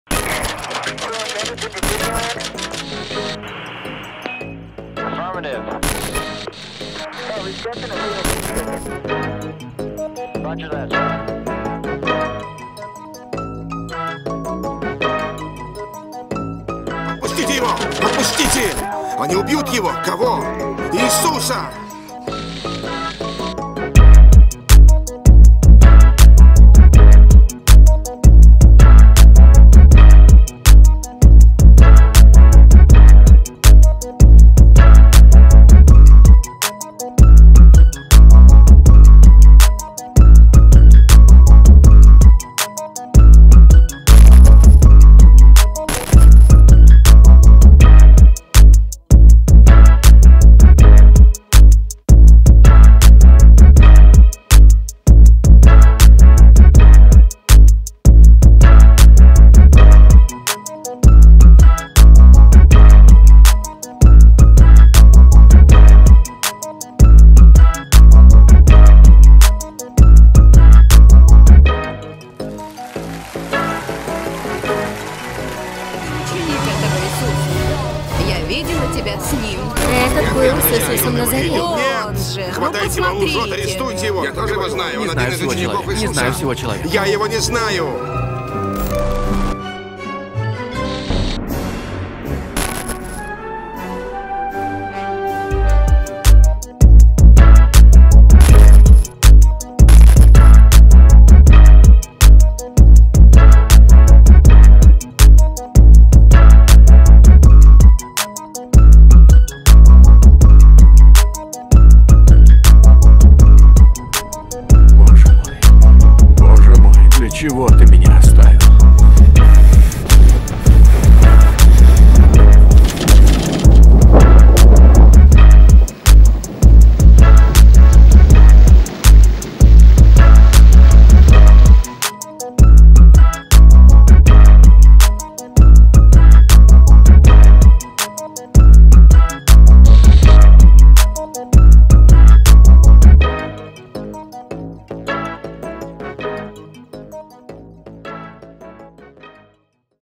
Апостол... трус или предатель?
Есть ли оправдание трусости? особенно если ты предал... Бога?